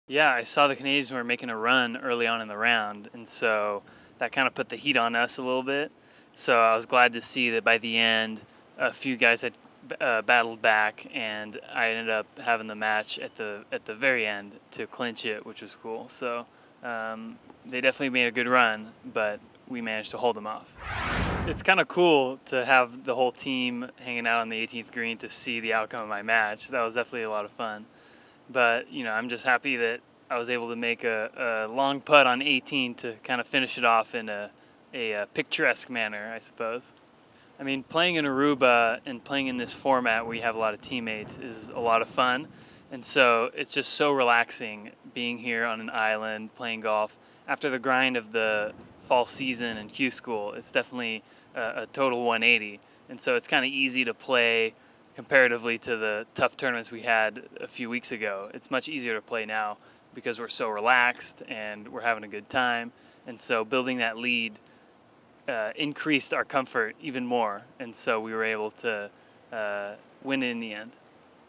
Martin Trainer interview after the PGA TOUR Latinoamérica victory at the Aruba
Following his 2-up win over Michael Gligic on the final day of the 2016 Aruba Cup, Martin Trainer talks about earning the point that secured the victory for Team PGA TOUR Latinoamérica at the 2016 Aruba Cup.